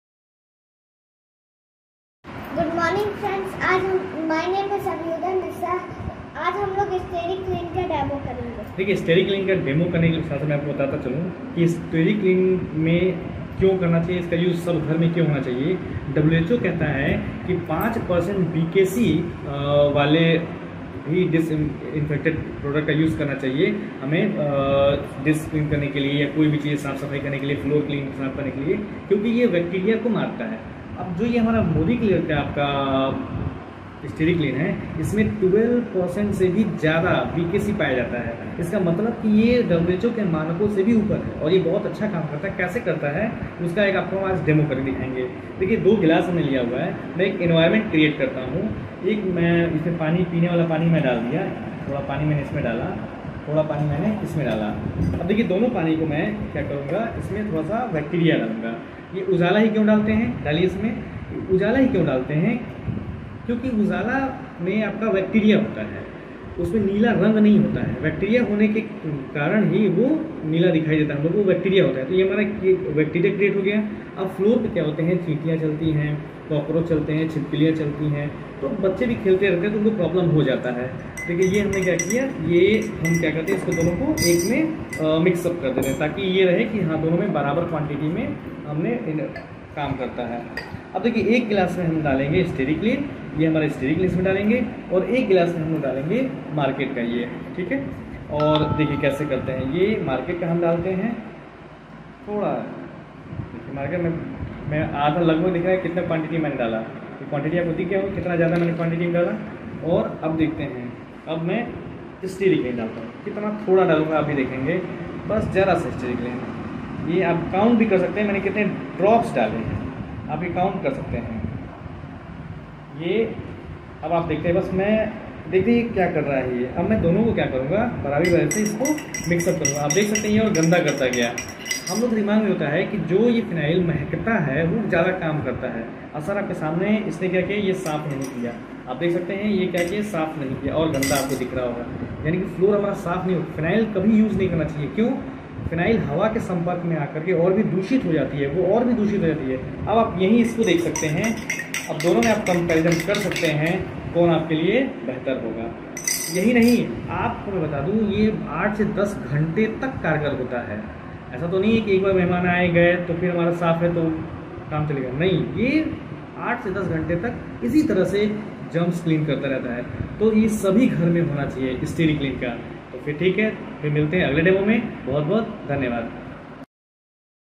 STERI CLEAN DEMO
STERI CLEAN फ्लोर क्लीन, बैक्टीरिया क्लीन करने के लिए एक सर्वोत्तम विकल्प है। इसमें किसी भी तरह का कोई भी हानिकारक केमिकल नहीं है । इसमें 12% से भी ज्यादा BKC पाया जाता है।ये WHO की मात्रक को भी पूरा करता है । जिनको भी चाहिए यह मोदी केयर फैमिली में मिल जाएगा। मोदी केयर का यह बहुत ही बढ़िया उत्पाद है। मोदी केयर से जुड़ने के लिए आप संपर्क कर सकते हैं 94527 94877 पर।